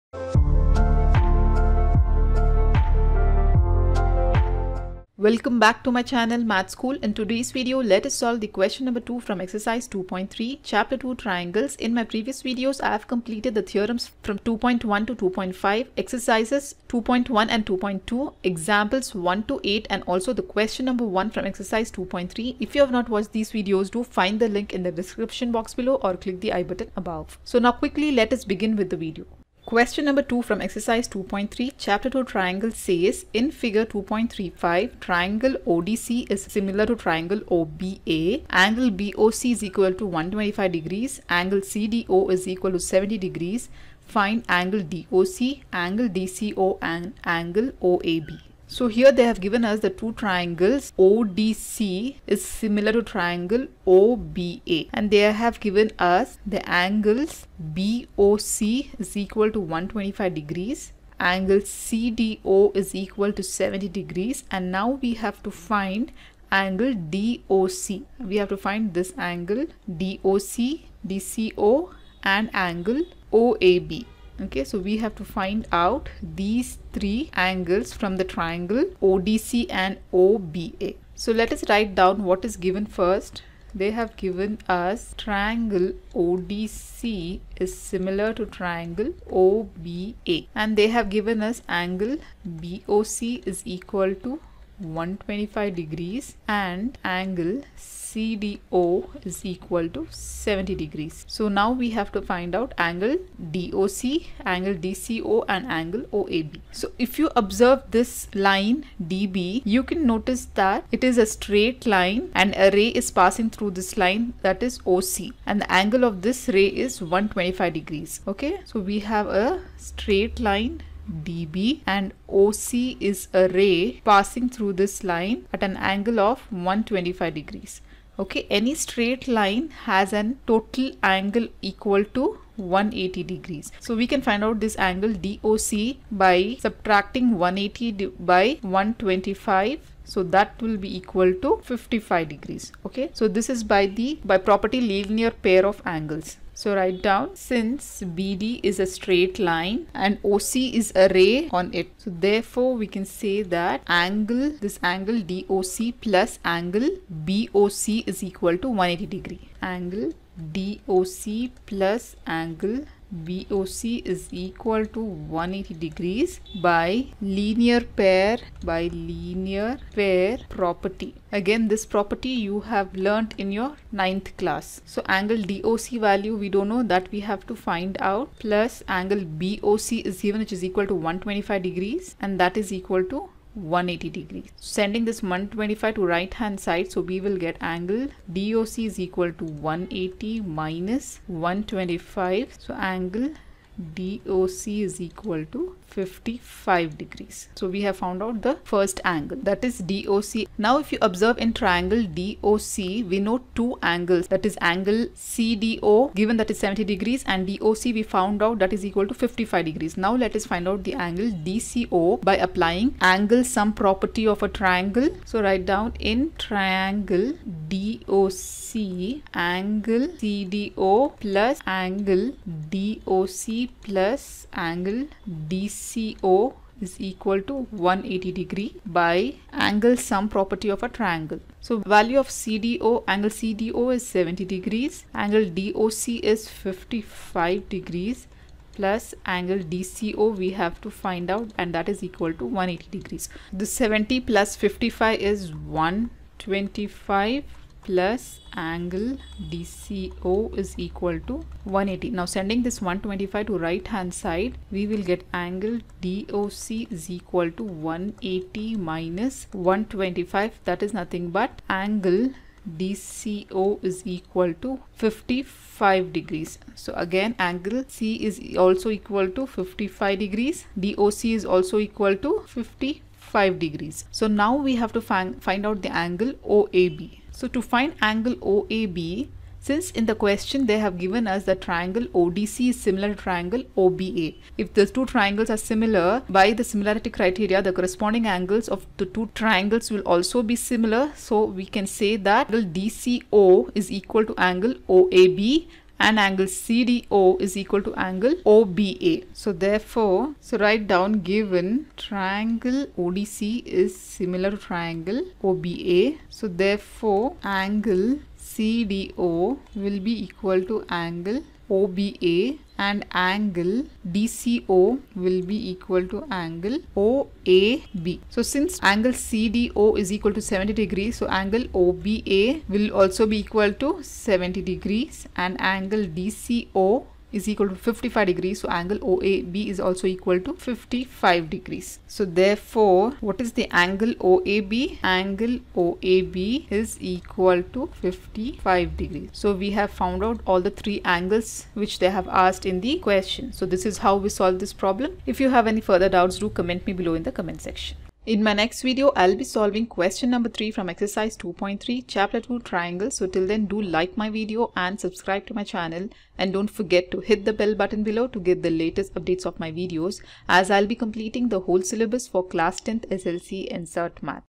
Triangles | class 10 chapter 2 Exercise 2.3 Question 2 | Karnataka Board SSLC NCERT Math 2020
In this video I will be covering chapter 2 Triangles Exercise 2.3 Question 2. Hope you will take advantage of this video to understand and learn the Problems easily.

 chapter 2 Triangles - Exercise 2.3 Question 2